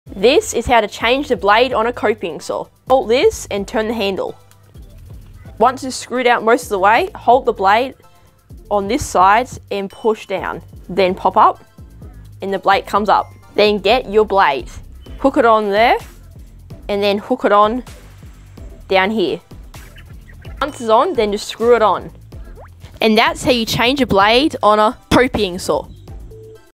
How to Change the Blade on a Coping Saw - The EASIEST Way EVER!
In this video I show you how to change the blade on a coping saw!😀😃